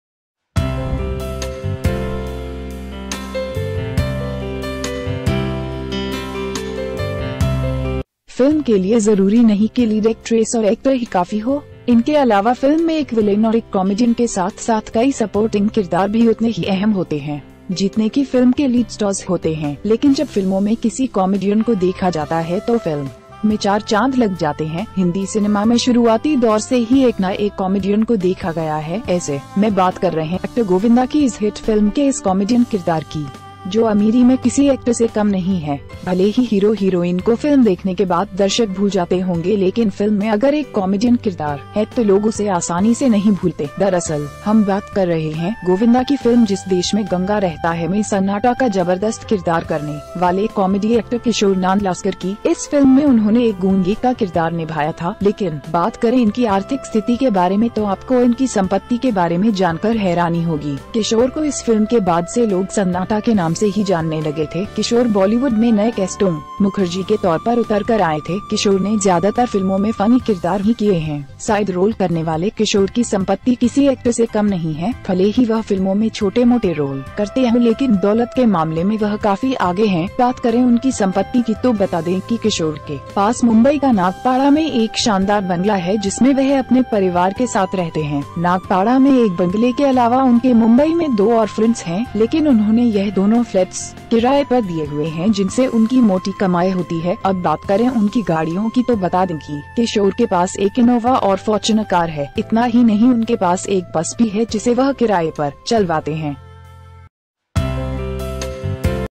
गोविंदा की फिल्म का ये गरीब सन्नाटा हे करोडो का मालिक, संपत्ति जानकर उड़ जाएंगे होश
Not necessarily a lead actress and an actor for the film, apart from this, in the film a villain and a comedian, along with many supporting characters are equally important as the film's lead stars. But when a comedian is seen in the films, then there are four moons in the film. A comedian has been seen since the early stages of Hindi cinema. Actor Govinda is talking about this comedy character of this hit film ... which is not less than an actor in the emi ...
Even if the hero-heroine has forgotten the audience after watching the film, but if there is a comedian character in the film, then people do not forget it easily. Actually, we are talking about In the country of Govinda's film Ganga lives in, the teenage actress Katar Nandlaskar, who played the role of SUNATA, has a great role. In this movie he played a dumb character. But talk about their financial condition, you will be surprised to know about their property.
Since Kishore's film, people started to know only by the name of silence. Kishor came out as a new Casto Mukherjee in Bollywood. Kishor has played only funny characters in most of the films.
The teenage property of a side roll is less than an actor. Even if she does a little bit of rolls in films but she is quite ahead in the matter of wealth. Talk about their property so that the teenager has a magnificent bungalow in Nagpada, Mumbai, in which he lives with his family.
Apart from a bungalow in Nagpada, there are two more flats in Mumbai. But they have given both flats on rent which they earn. Now talk about their trains and tell them that the teenager has an Innova and Fortune car. Not only that, they also have a bus which they hire.